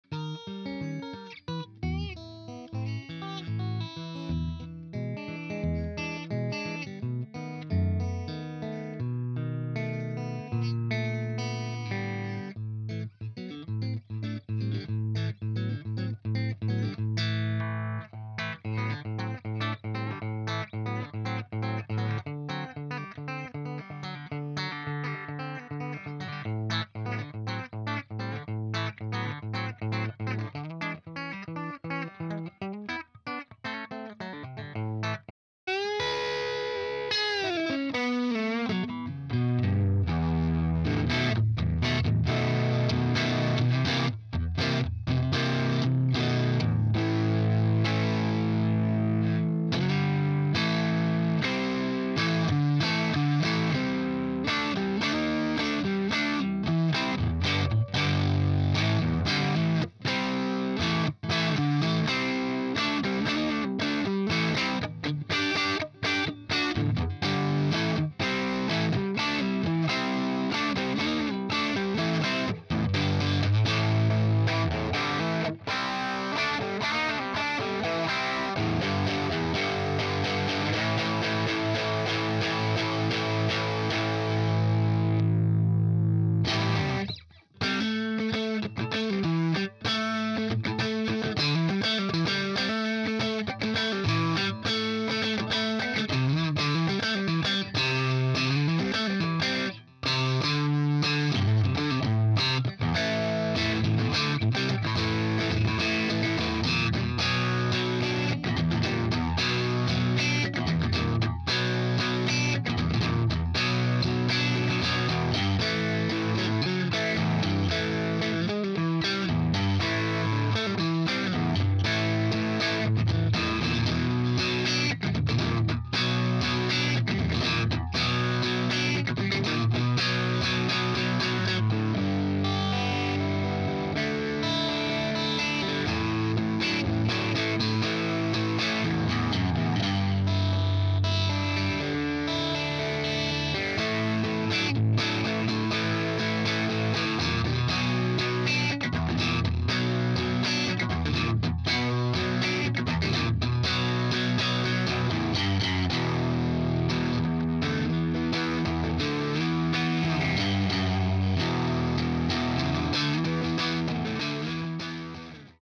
My mate Brian jamming on my Siggi Braun through H&K Puretone
so my friend was just swinging by and started jamming on my lovely Siggi Braun Custom Shop Tele.
I couldn't help but randomly throw an sm57 in front of the speaker...
Literally just used whatever settings the amp was on and  the position I happened to put the 57 in, just eyeballing it..zero post processing whatsoever, but I think it's still quite obvious what the combination Brian-Siggi-Puretone is capable of.